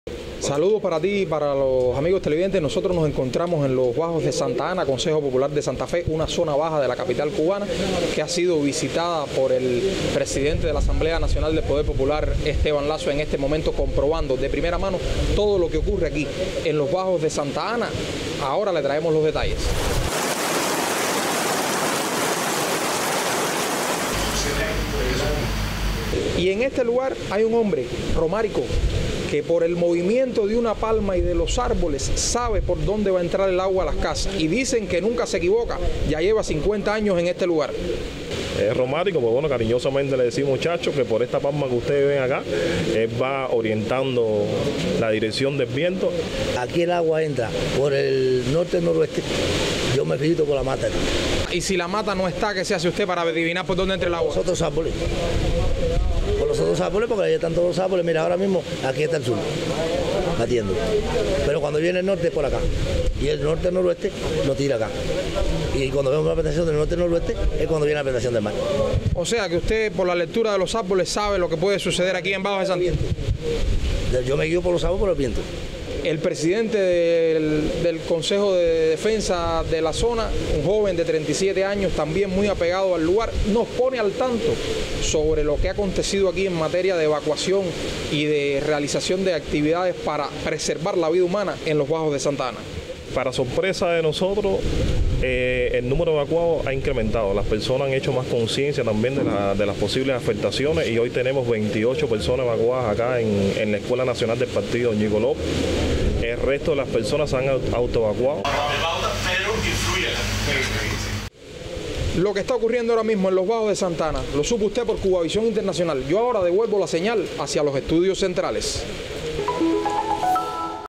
Cuba - Autoridades de la capital recorrieron zonas afectadas de La Habana a causa del huracán
El Presidente de la Asamblea Nacional del Poder Popular Esteban Lazo y autoridades de la capital recorrieron la comunidad de Bajos de Santa Ana, perteneciente al municipio Playa en la capital cubana. El periodista Duanys Hernández está en el escenario y tiene los detalles.